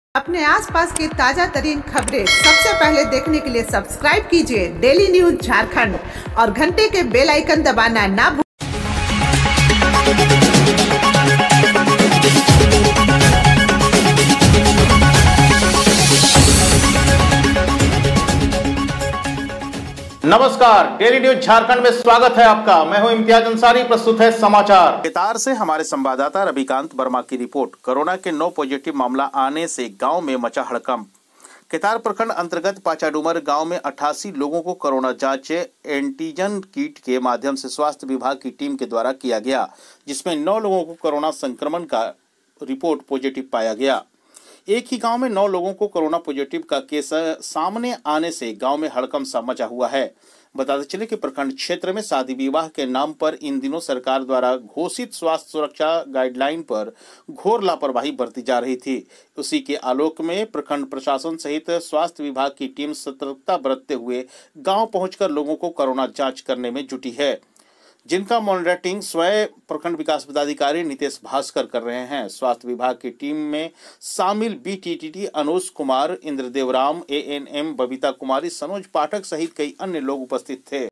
केतार।।कोरोना के 9 पॉजिटिव मामला आने से गांव में मचा हड़कंप | Daily News Jharkhand
केतार से रविकांत वर्मा का रिपोर्ट

**कोरोना के 9 पॉजिटिव मामला आने से गांव में मचा हड़कंप**

केतार :केतार प्रखंड अंतर्गत पाचाडुमर गांव में 88 लोगो को कोरोना जांच एंटीजन कीट के माध्यम से स्वास्थ्य विभाग की टीम के द्वारा किया गया। जिसमें 9 लोगों को कोरोना संक्रमण का रिपोर्ट पॉजिटिव आया है। एक ही गांव में 9 लोगों को कोरोना पॉजिटिव का केस सामने आने से गांव में हड़कंप सा मचा हुआ है। बताते चलें कि प्रखंड क्षेत्र में शादी विवाह के नाम पर इन दिनों सरकार द्वारा घोषित स्वास्थ्य सुरक्षा गाइडलाइन पर घोर लापरवाही बरती जा रही थी। उसी के आलोक में प्रखंड प्रशासन सहित स्वास्थ्य विभाग की टीम सतर्कता बरतते हुए गांव पहुंचकर लोगो को कोरोना जांच करने में जुटी है। जिसका मॉनिटरिंग स्वयं प्रखंड विकास पदाधिकारी नितेश भास्कर कर रहे हैं। स्वास्थ विभाग की टीम में शामिल बीटीटी अनुज कुमार,इंद्रदेव राम,एएनएम बबीता कुमारी, मनोज पाठक, सहित अन्य लोग उपस्थित थे।